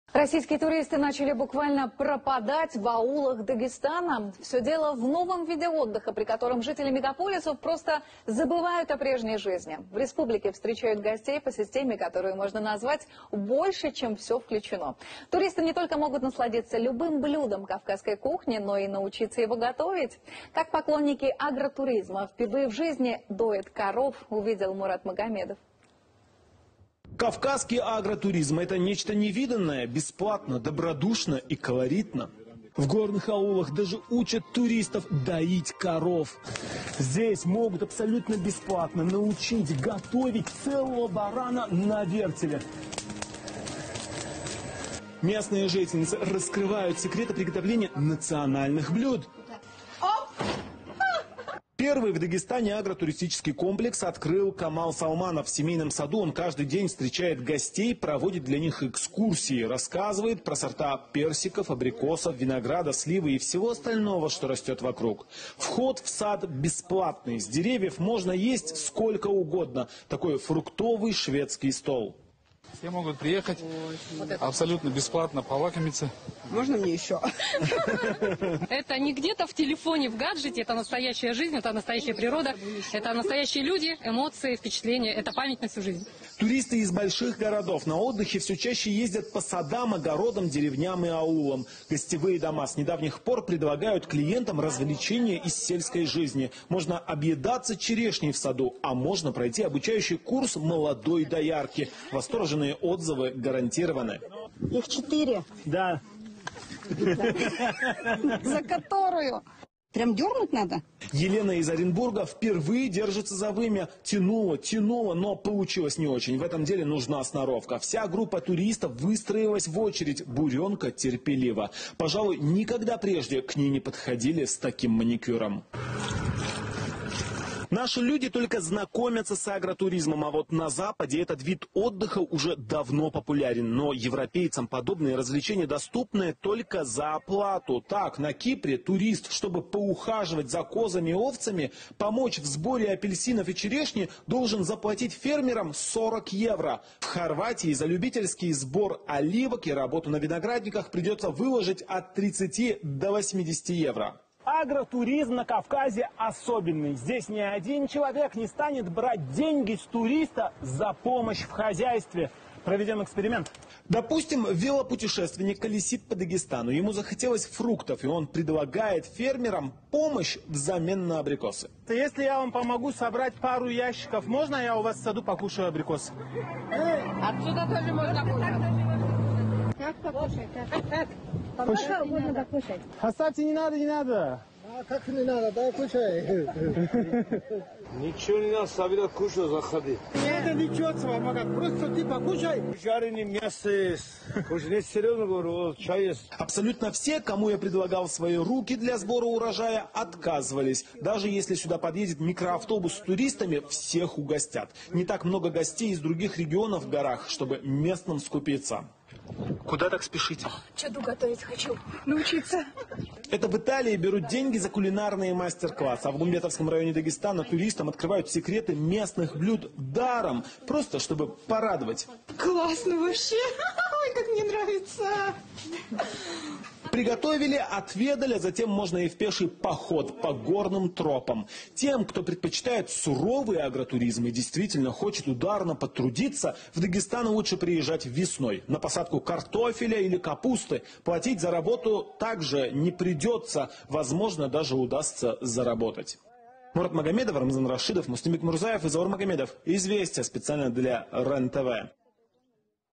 Нескучный сад Салмановых. продолжаем радовать наших гостей. Сюжет телеканала Рен
Мы всегда, вот уже на протяжении 35 лет, принимали гостей в нашем саду, но с 2017 года начали принимать туристов из других регионов и запустили наш сад в формате агротуристического проекта. После того как мы начали продавать персики онлайн, это привлекло внимание федеральных каналов и блогеров и к нам на этот раз приехал в гости телеканал Рен :). Дальше смотрите сами :))